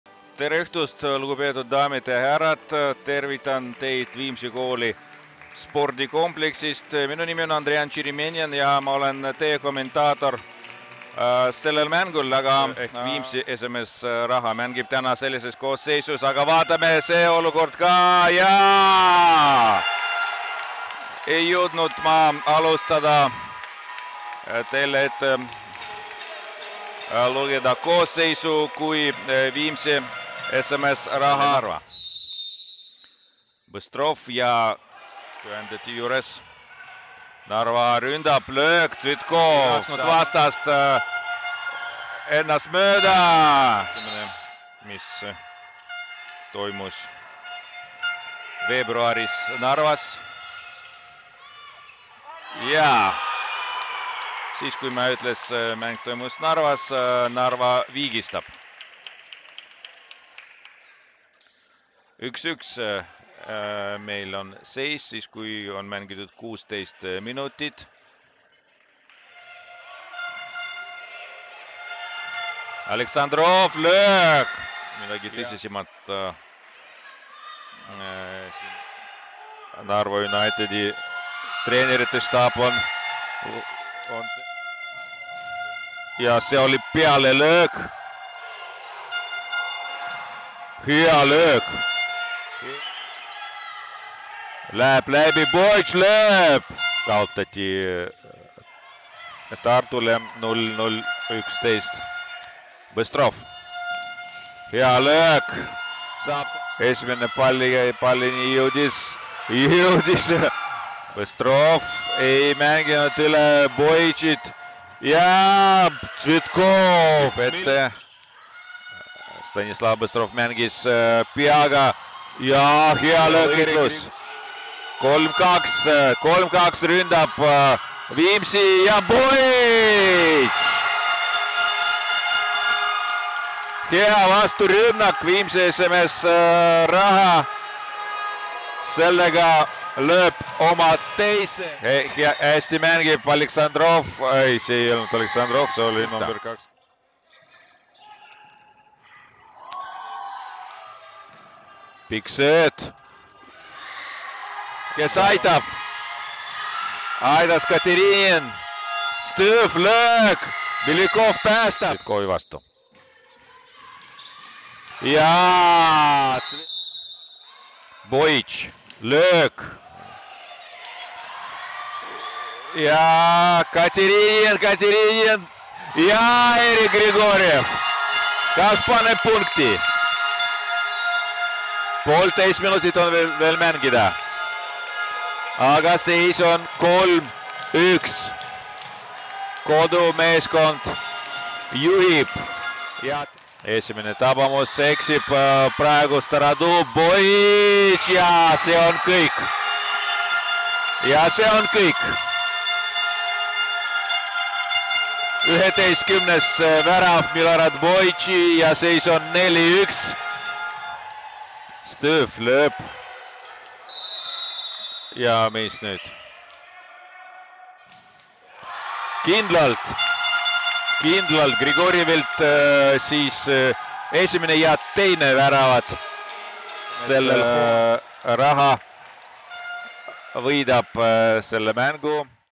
Viimsi FC Smsraha - Narva United FC (25.11.2023) I 5:1 I Coolbet saaliliiga 6. voor
Vooru lõppakord mängiti Viimsi kooli spordikompleksis, kus Viimsi FC Smsraha võttis Narva United FC üle 5:1 võidu.